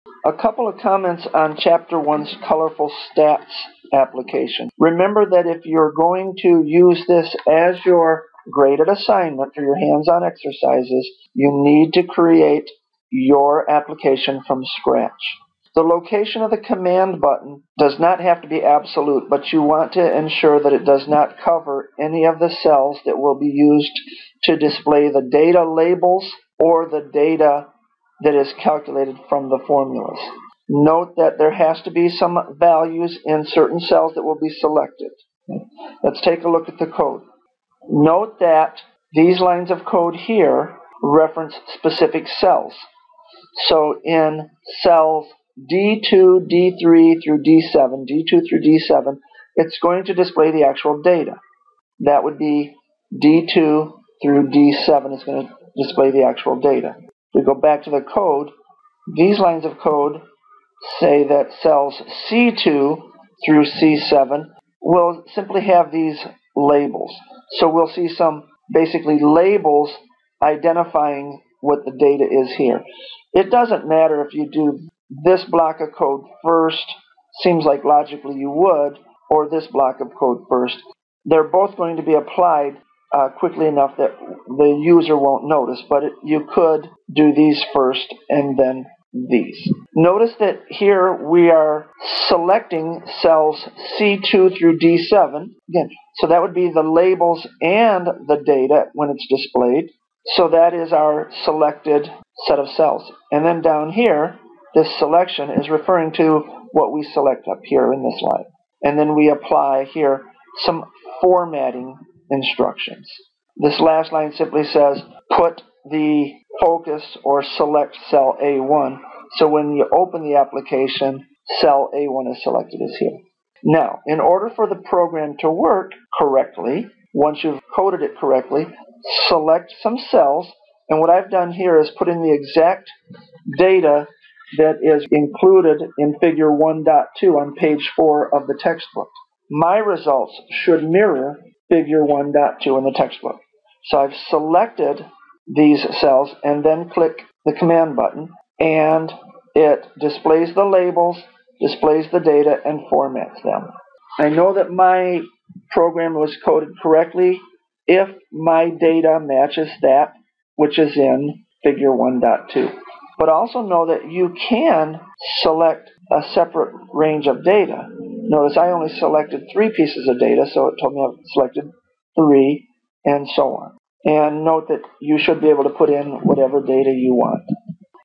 VBA Excel Selecting and Formatting Ranges
Use Excel VBA to select ranges and use functions and formatting.